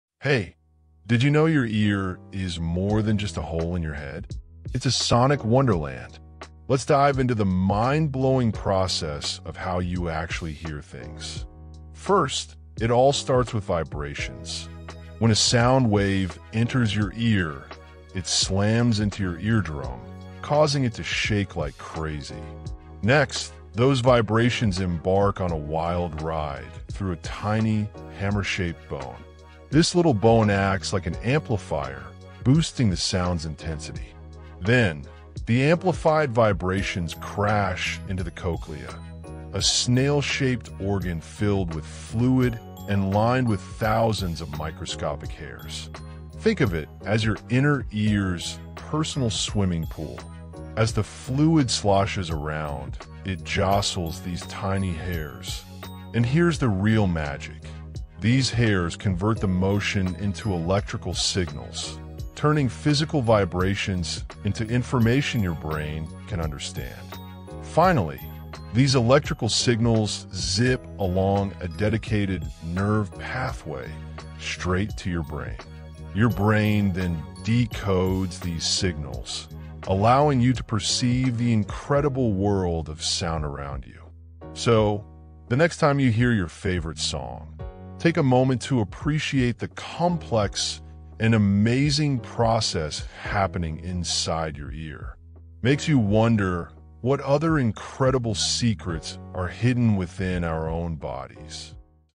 🤯 Sound's Secret Life: Journey Inside Your Ear!
Ever wondered how you actually HEAR? 👂 Discover the incredible process of sound as it travels through your ear! From eardrum vibrations to brain interpretation, see the magic unfold! ✨

🟦 KEY FEATURES:
1️⃣ Eardrum Vibration: The start of the sound journey.
2️⃣ Tiny Bones Amplification: How sound gets louder.
3️⃣ Microscopic Hair Cells: Converting motion to electrical signals.
4️⃣ Brain Interpretation: Decoding the sound you hear.

🟩 WHY IT MATTERS:
- Understand the complexity of hearing.
- Appreciate the delicate mechanisms of the ear.
- Gain insights into potential hearing issues.